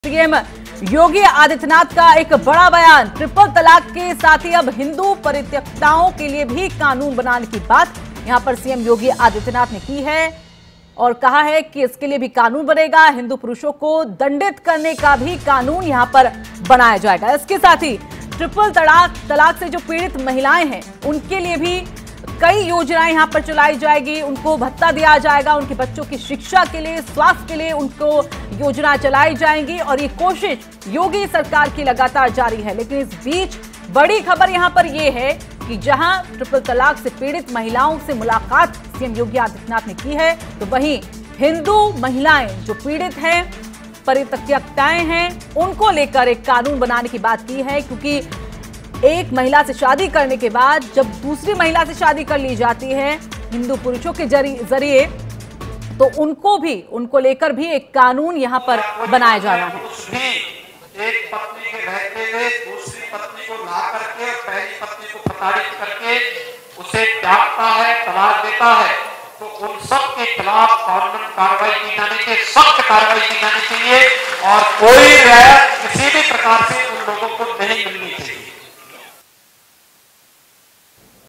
Triple तलाक पर बैन के बाद अब हिंदू महिलाओं के लिए बड़ा ऐलान
उत्तर प्रदेश के मुख्यमंत्री योगी आदित्यनाथ ने ट्रिपल तलाक की पीड़िता महिलाओं से मुलाकात की. इस दौरान सीएम योगी ने कहा कि हिंदू परित्यक्ता महिलाओं को भी ऐसा ही न्याय दिलाया जाएगा. एक शादी कर दूसरी महिला को रखने वाले हिंदू पुरुषों को दंडित करने का कानून बनेगा.